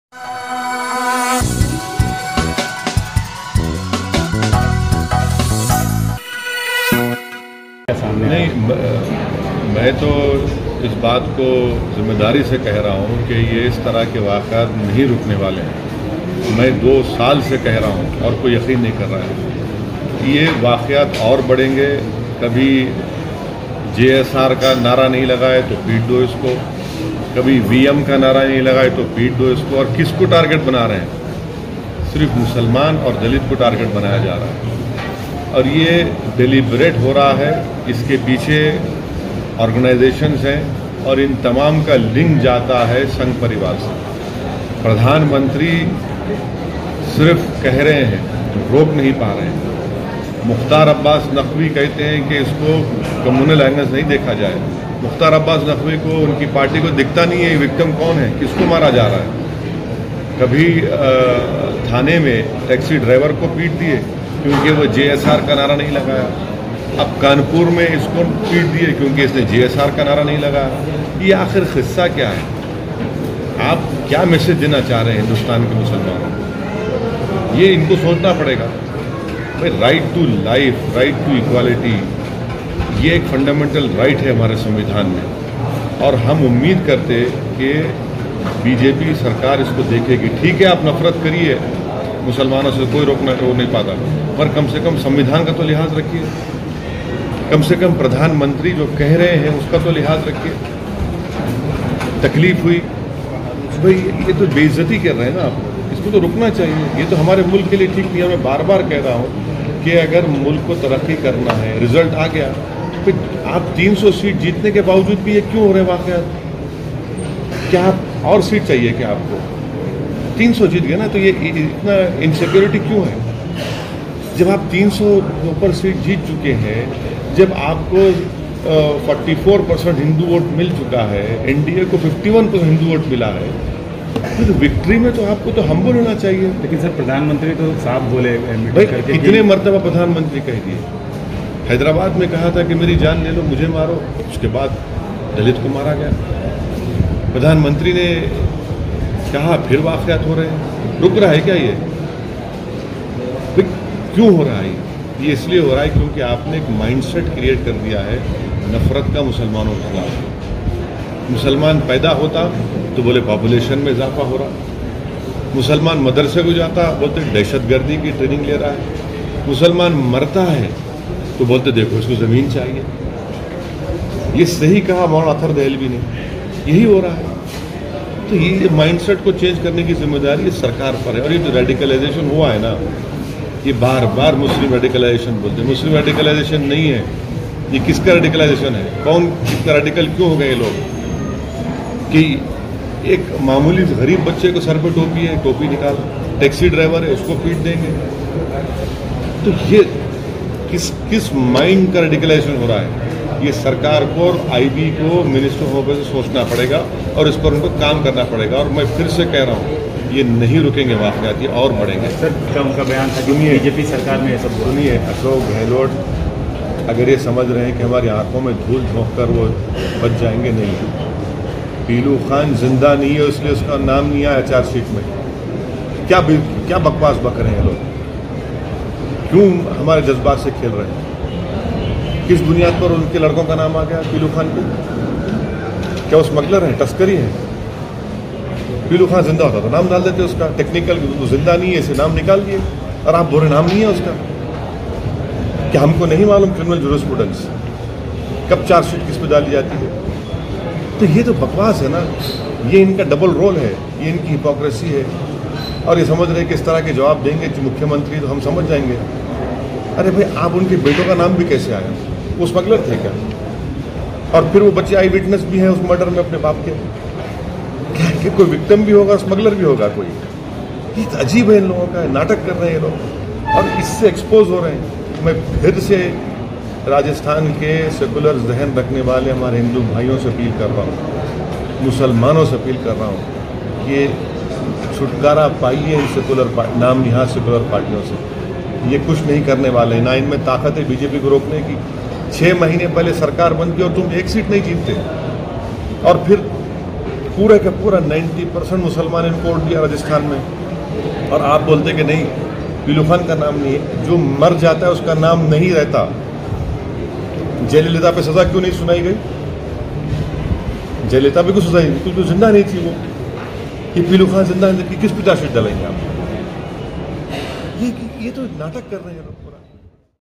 Asaduddin Owaisi Says Mob lynchings of Muslims & Dalits are linked to Sangh Parivar | Overseas News
Asaduddin Owaisi Says Mob lynchings of Muslims & Dalits are linked to Sangh Parivar